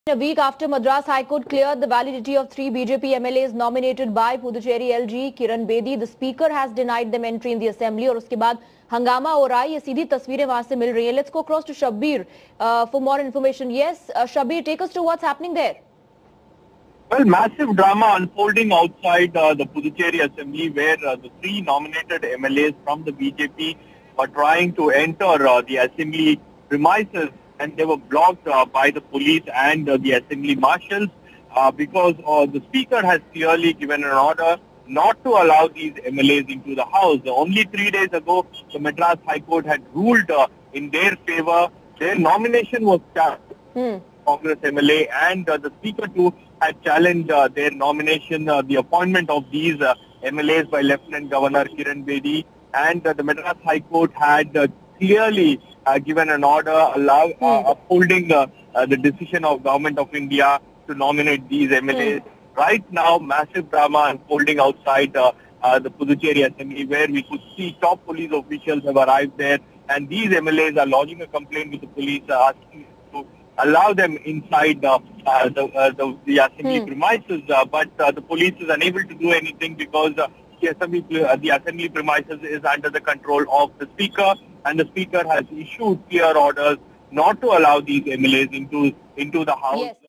Puducherry: 3 BJP MLAs protest outside assembly
Three nominated BJP MLAs V Saminathan, KG Shankar and S Selvaganapathy protest outside Puducherry Legislative Assembly after they were allegedly stopped by the speaker from entering the assembly.